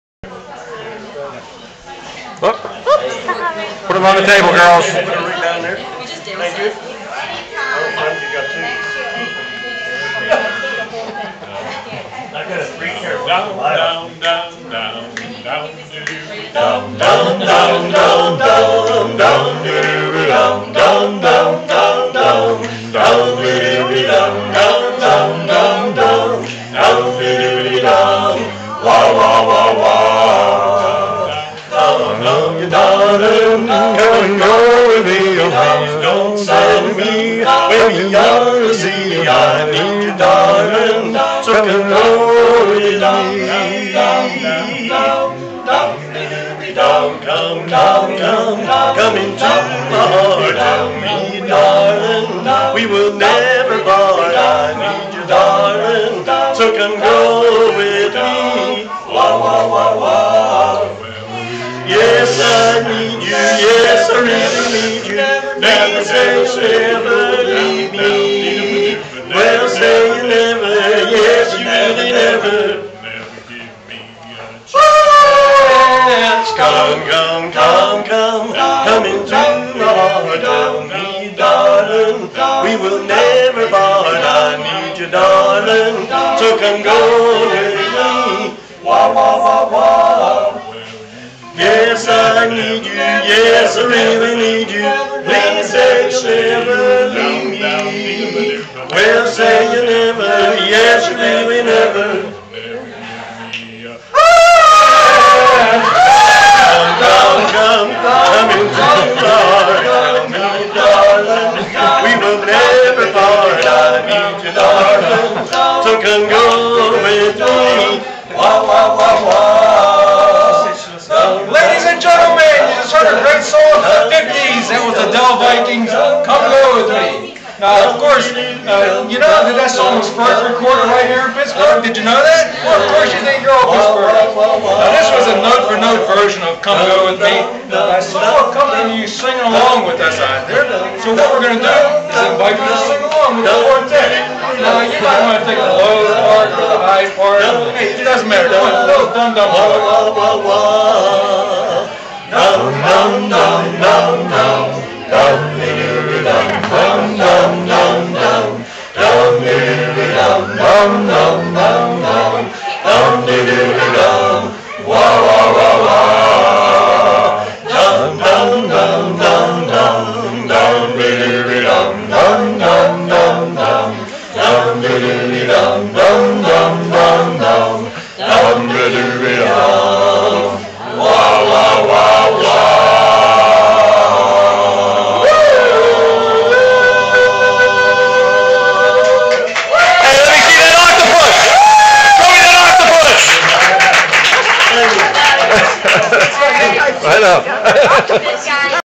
bshop_quartet_5.MOV
The Barbershop Quartet visits ESSpa Kozmetika Organic Skincare during the 2008 Aspinwall Light Up Night to kick off the Christmas Holidays in Pittsburgh, PA